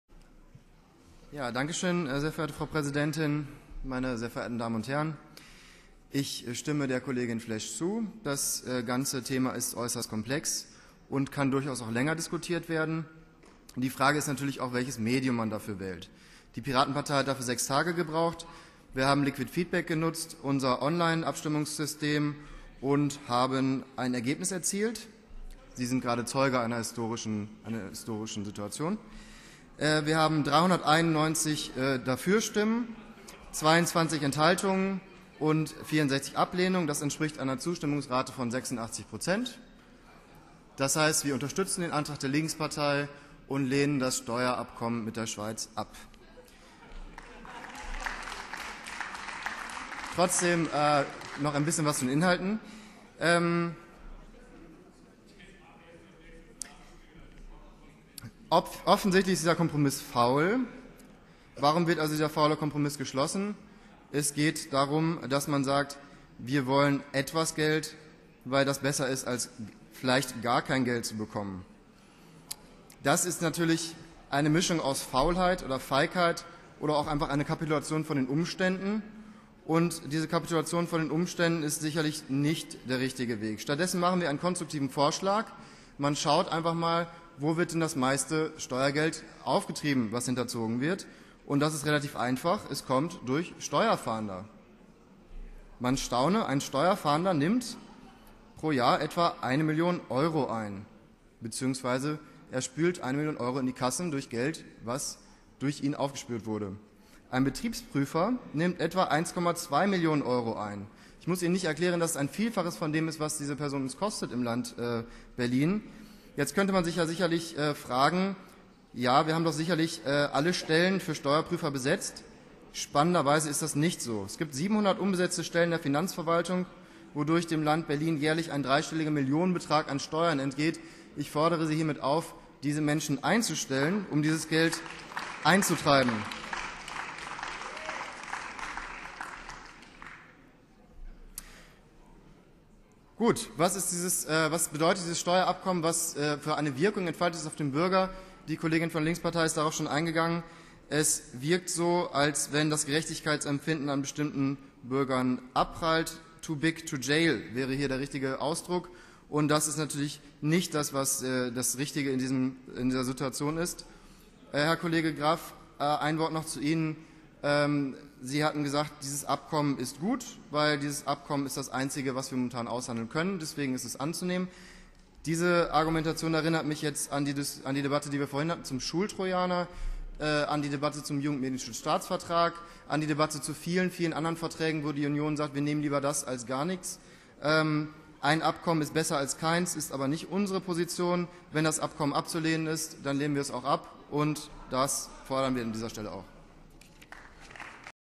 10.11.2011 - Abgeordnetenhaus Berlin - Schwarzgeldabkommen mit Schweiz - Fabio Reinhardt (Piraten)
Schwarzgeldabkommen mit Schweiz
Die Linke bringt einen Entschließungsantrag in das Abgeordnetenhaus ein, in dem der Senat aufgefordert wird, das Schwarzgeldabkommen mit der Schweiz im Bundesrat abzulehnen. Die Abgeordneten beschließen, es in den Ausschüssen weiter zu diskutieren.

Fabio Reinhardt (Piraten) spricht von einem offensichtlich "faulen Kompromiss". Bei dem Abkommen habe man vor den Umständen kapituliert. Der richtige Weg sei, mehr Steuerfahnder einzusetzen. Momentan seien in Berlin 700 Stellen in den Finanzverwaltungen unbesetzt, wodurch dem Land ein dreistelliger Millionenbetrag verlustig gehe.

10.November 2011
2. Sitzung des Berliner Abgeordnetenhauses